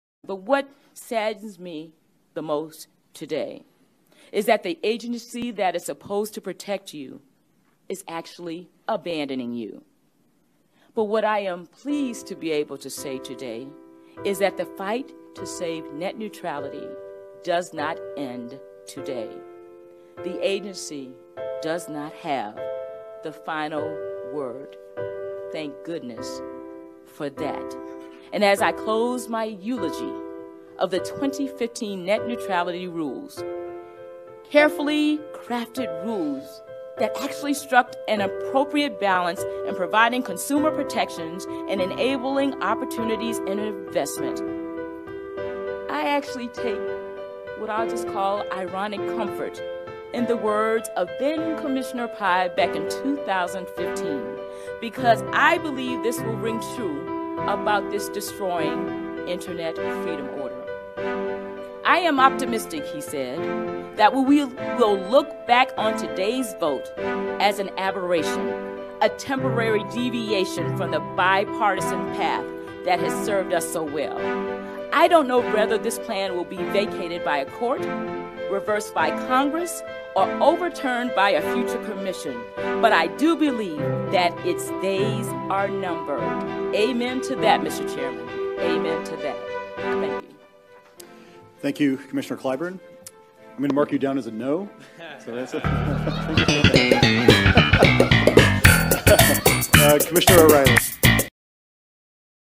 Ajit Pai 😺 loves Ajit Pai 😻
Sorry for the Movavi-free-trial water-marks throughout the video, as you can see I'm using a free-trial program to edit this video. Link to the original video and song, is down below.

Context of the video: Mignon Clyburn gives a strong motivational speech about the repeal of Net-Neutrality, but gets ignorantly dismissed by apparently the funniest joke ever told by anyone!